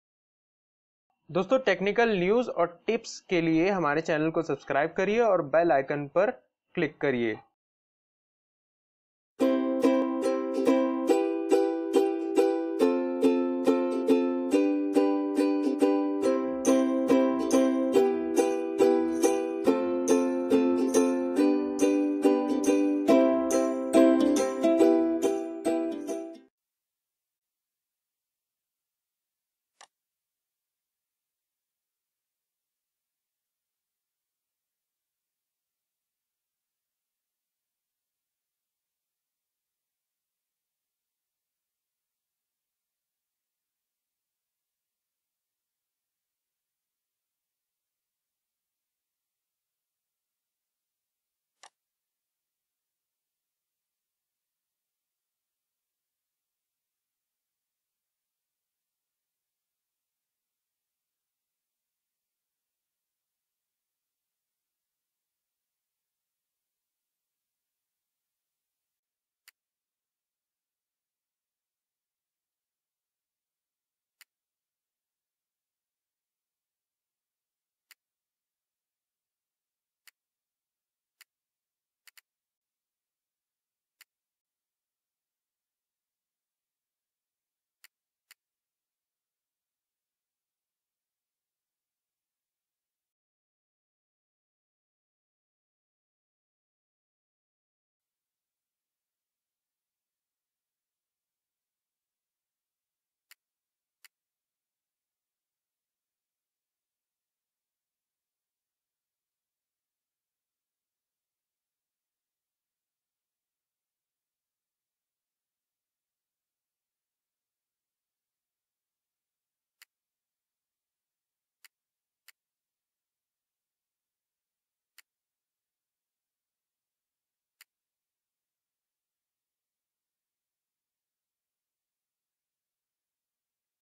How to install Shareit on PC (windows & Mac)? #4 Help & Tips
hello dosto aj mai is video mai apko dikhaonga ki kiss tarah se app shareit download or install kr sakte ho pc (windows & Mac) me.

Best alternate of youtube? all things about IGTV explain in Hindi H&T"